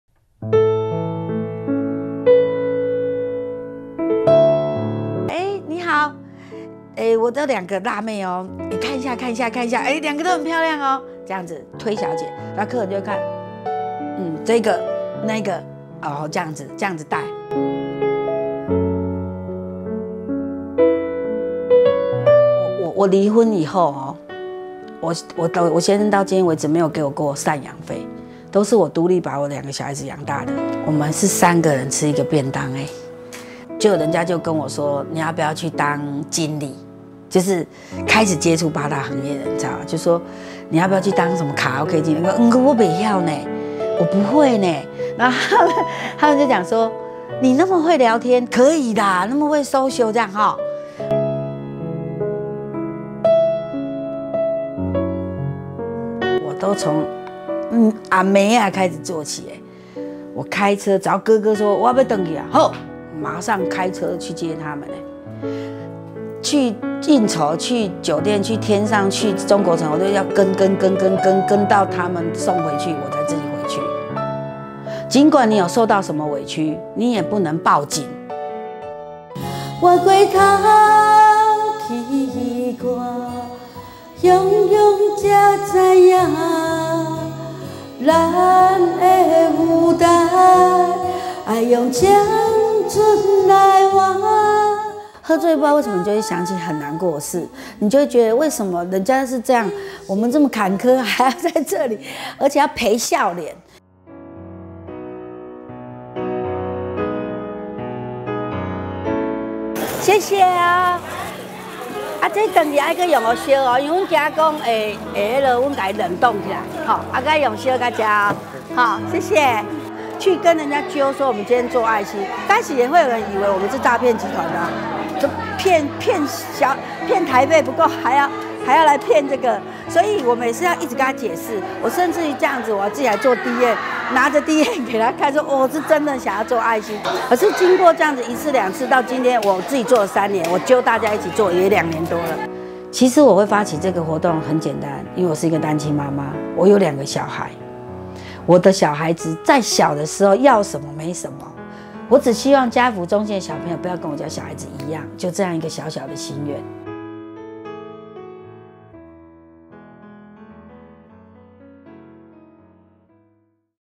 【台灣壹週刊】傳播妹的媽媽桑，為何默默做這事？
傳播妹，是一個引人非議的行業。謝慧謹卻從不隱瞞自己是傳播妹的老闆，她認為不偷不搶，沒什麼好避諱的。她甚至帶著旗下小姐到處做公益，她說：「傳播妹1小時檯費1千元，真的比一般人賺得多，是應該付出一些啊。」每個公益活動結束後，她和傳播妹最殷殷期盼的，就是社福團體頒發的感謝狀，就算因為工作常被人羞辱、看不起，至少這一張張感謝狀，讓她們看到自己的一點價值和尊嚴。